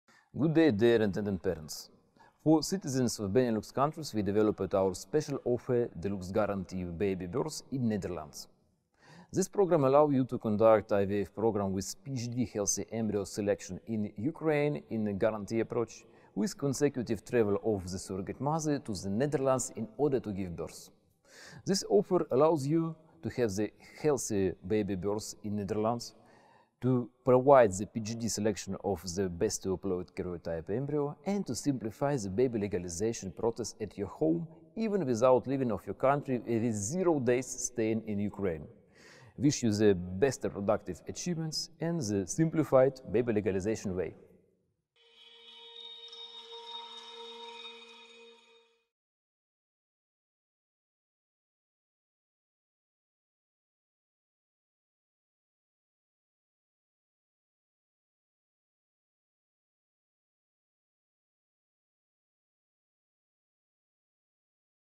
Deluxe Guarantee package in the Netherlands - Feskov HRG clinic
Offer for couples who want an expanded surrogacy program with childbirth in their country.

Feskov HRG Clinic offers various options and ready-made service packages for couples, including childbirth abroad. The Deluxe Guarantee package in the Netherlands is an offer that contains the entire medical part of the program, extended genetic testing for the birth of a healthy child, all payments to the surrogate mother and donor (if necessary), as well as guest services for the couple from the first arrival to the end of the program.

We pay special attention to legal services in the Deluxe level packages - this package for the child and the couple includes the registration of the newborn child in the civil registry «Gemeente», and the registration of BSN for non-residents of the Netherlands.

The package offers the selection of an egg donor phenotypically similar to the future mother: all possible phenotypes, including exotic ones, are collected in the database of our clinic. Another nice bonus is the opportunity to choose the gender of your child.

No waiting lists - the program starts immediately after payment and provides coverage for all risks (in case of unsuccessful IVF or abortion, we will repeat the entire cycle at our own expense an unlimited number of times until your healthy baby is born).

Feskov Human Reproduction Group – Parenthood happiness for those who lost hope!